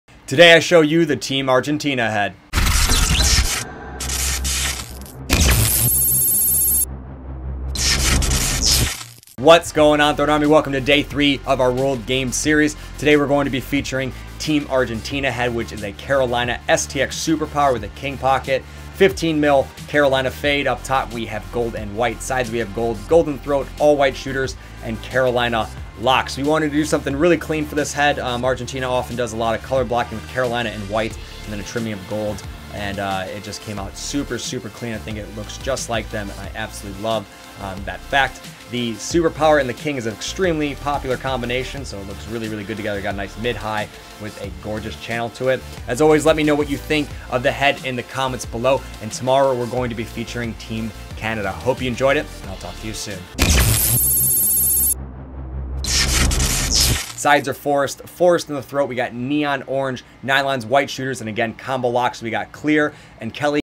World Games Series: Argentina | STX Super Power with King Pocket
Joe strings up an Argentina themed carolina blue STX Super Power with a King Pocket in 15mm carolina fade Throne Mesh!  Come meet the Throne crew at the Lax.com booth in Denver this week to pick up a piece of LE Camo. Joe and the guys will be there stringing sticks Monday, Tuesday, Thursday, and Friday!

Check out the latest and greatest Throne Mesh, Custom Stringing and Apparel